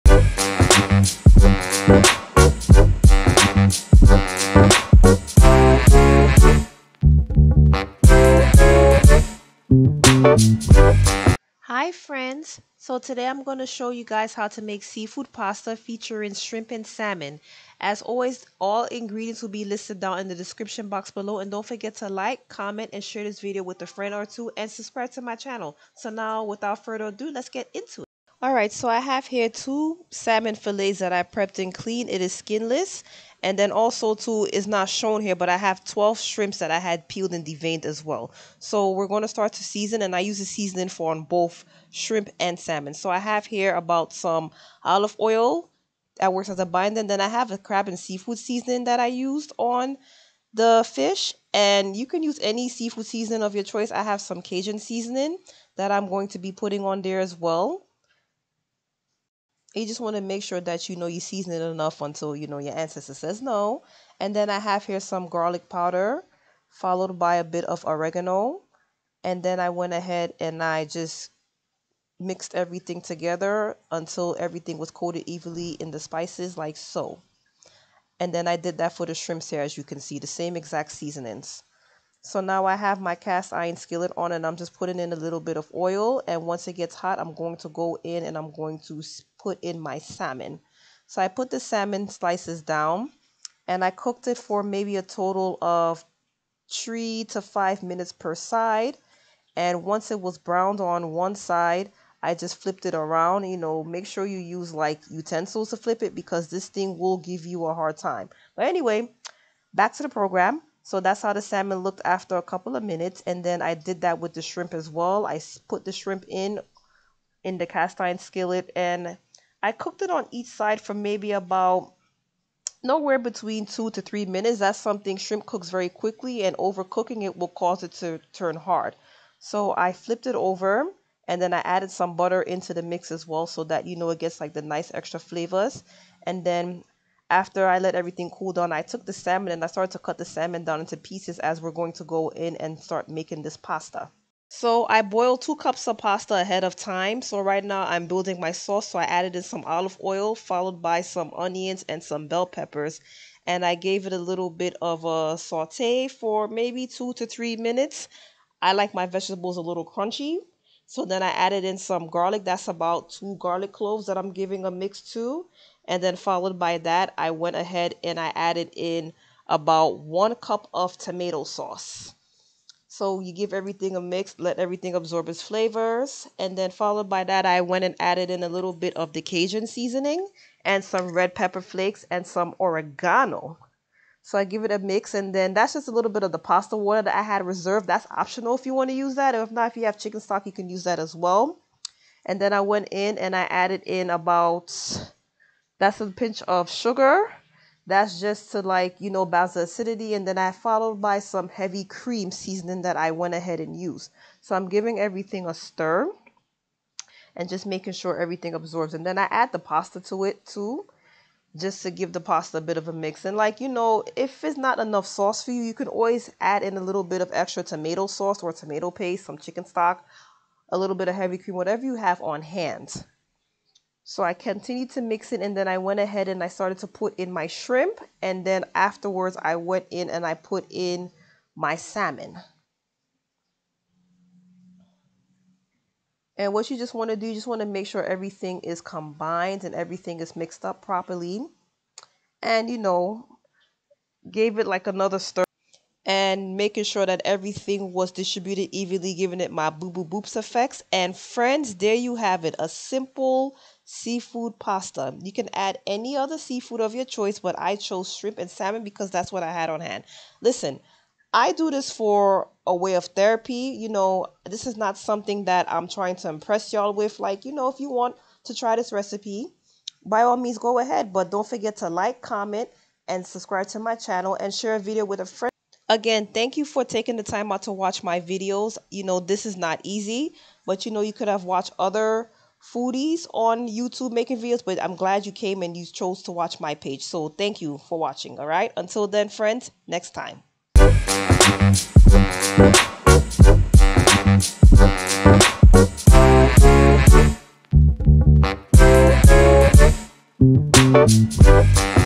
Seafood Pasta (with Shrimp and Salmon)
Hi Friends!  Check out my recipie for Seafood Pasta with Shrimp and Pasta.

Ingredients:
-2 Cups Pasta

-2 Salmon Filets
-12 Shrimp (peeled and deveined)

Seasonings:
-1 Tablespoon Olive Oil
-1 Tablespoon of Seafood Seasoning
-1 Tablespoon of Garlic Powder
-1 Tablespoon Cajun Seasoning
-1 Tablespoon of Oregano

**The above ingredients will be enough seasoning for both shrimp and salmon.  You can add seasonings more seasoning if you desire."

Sauce
-1 Cup of Tomato Sauce
-1/2 Cup of Heavy Cream
-Bell Pepper and Onion (how much you desire)
-2 Garlic Gloves
-1 Teaspoon Oregano
-1 teaspoon Cajun Seasoning
-Pinch of Sugar
-1/4 cup Pasta Water (optional)
-Pinch of Red Pepper Flakes or Cayenne Pepper
-Garnish with Parsley or Chives